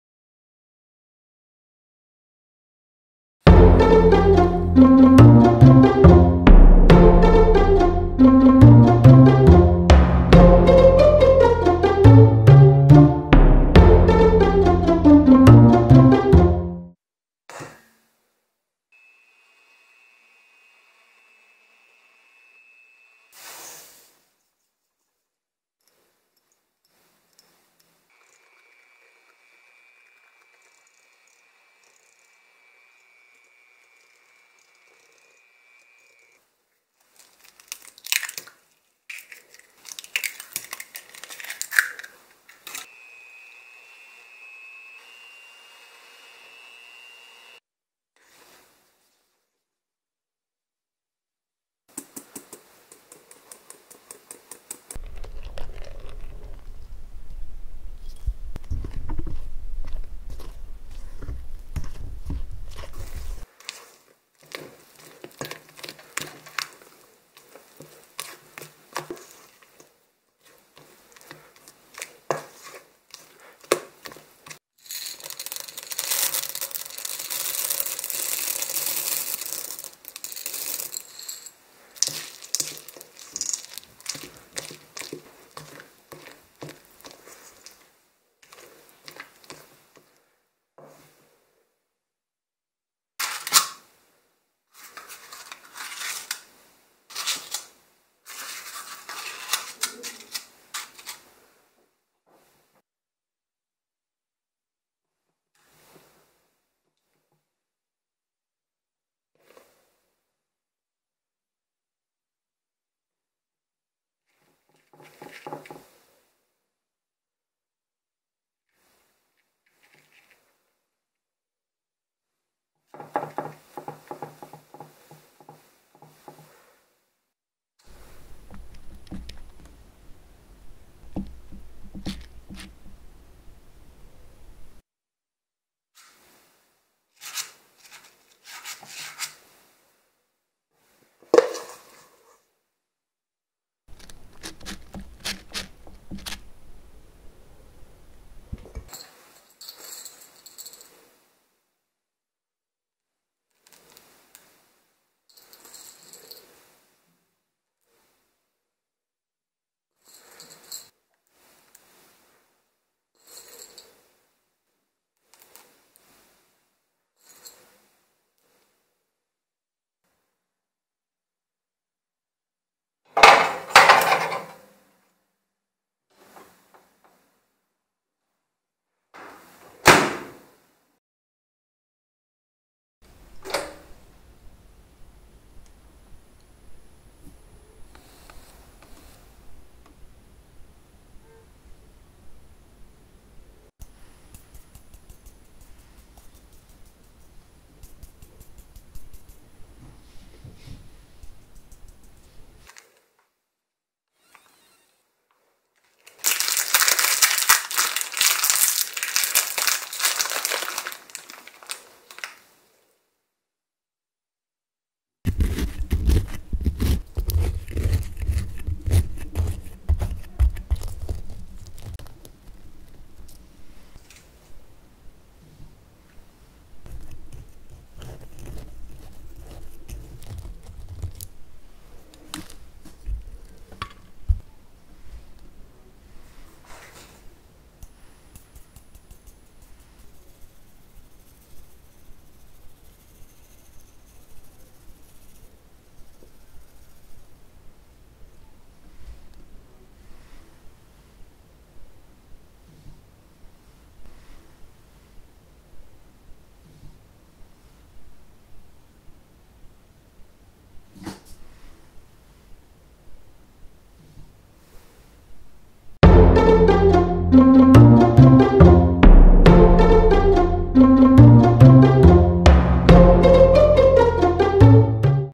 Chocolate muffin 초코머핀만들기~*
초코초코한 초콜릿 머핀을 만들었어요 ~*
초코칩이 듬뿍 들어가서 먹을때 초코칩이 씹혀서 좋았어요 ㅋㅋ
빵은 촉촉 폭신한 느낌이구요, 데코는 오레오로 해봤어요 ㅋㅋ
레시피 자체도 굉장히 쉽다는!
가루류 제외한 나머지 다넣고 섞다가 가루류 넣고 섞고 구으면 끝 !
틀이 없으시면 종이컵에 구우셔도 좋아요 :-D 계란빵 처럼요 !
다 구워지면 이쑤시개로 푹 찔러보아서 아무것도 묻어나는게 없으면 다 익은거에요 ~!

이 레시피는 많이 달지 않아서 어른들도 좋아하시더라구요 ㅋㅋ
요근래 친구 집들이 선물로 급히 구워갔었는데 호응이 좋아서 영상으로 찍어올려보아요~^-^
우유랑 먹으면 폭신폭신 달콤 궁합 굿 b

모두 초코초코한 스윗한 하루 보내세요~;-)